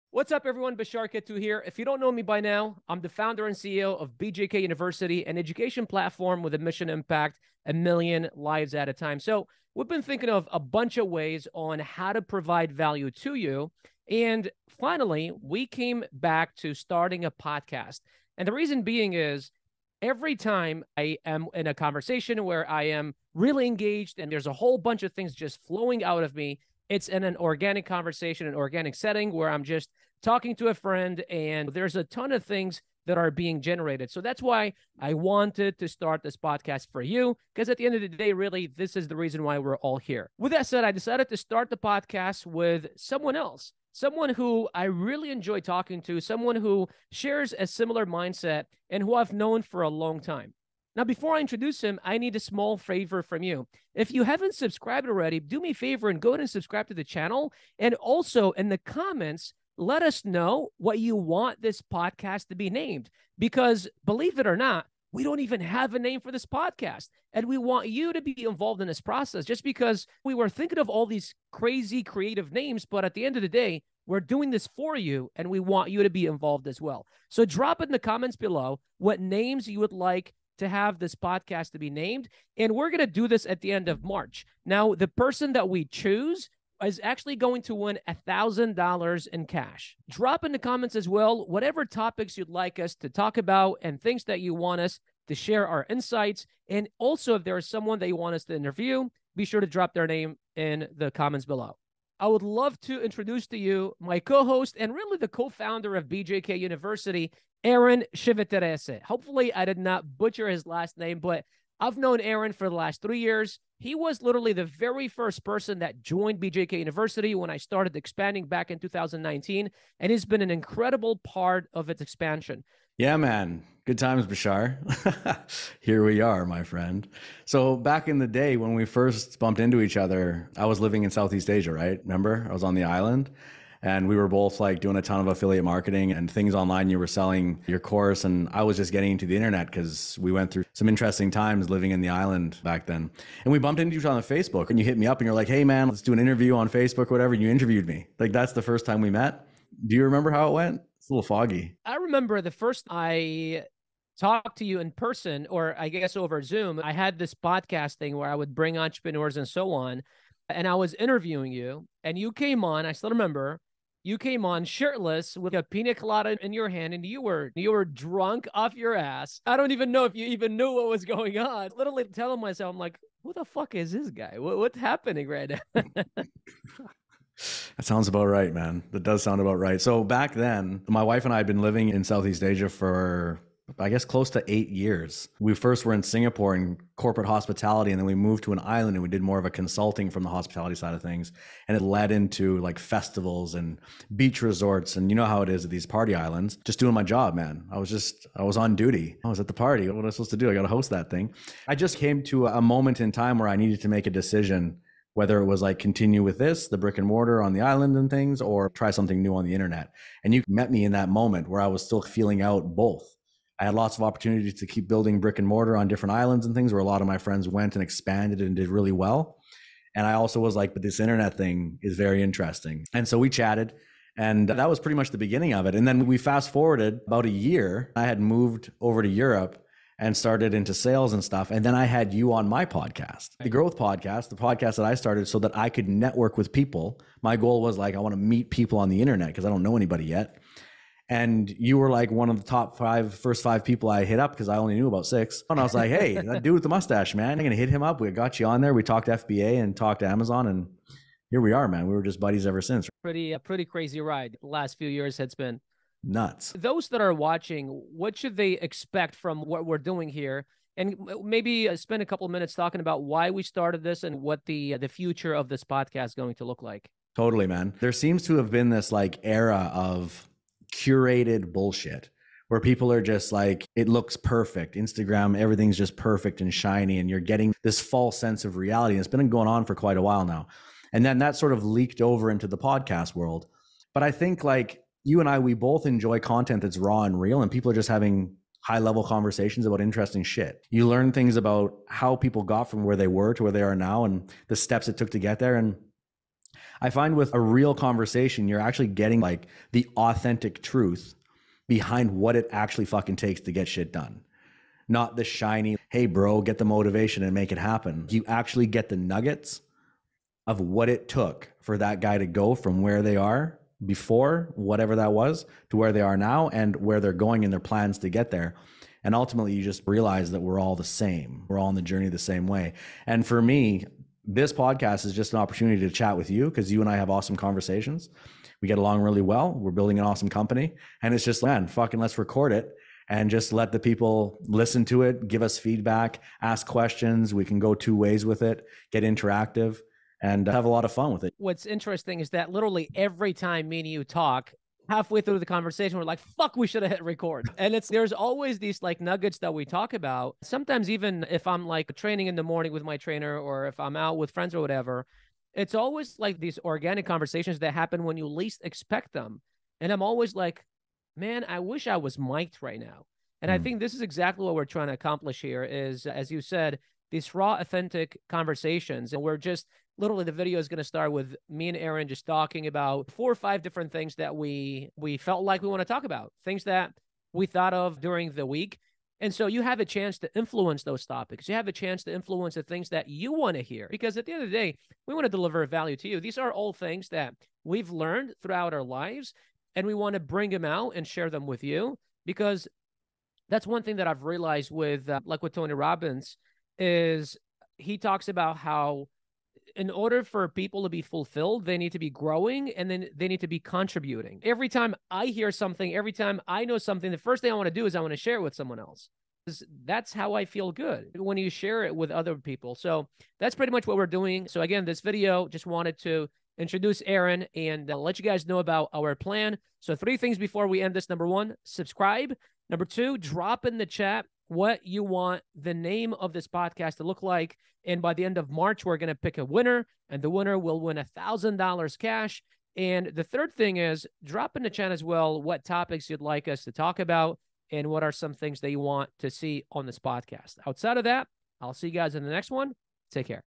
We Started a Podcast [$1k Giveaway]
Welcome to the new BJKU Podcast - We need your help to name this thing!

Also drop your ideas and wishes for topics you want us to talk about!

Get your Case Study: How to Crush It On Amazon from Someone Who ACTUALLY Sells On Amazon:

WE WANT TO HEAR FROM YOU:
Comment your questions below and let us know what you want us to cover in the next video.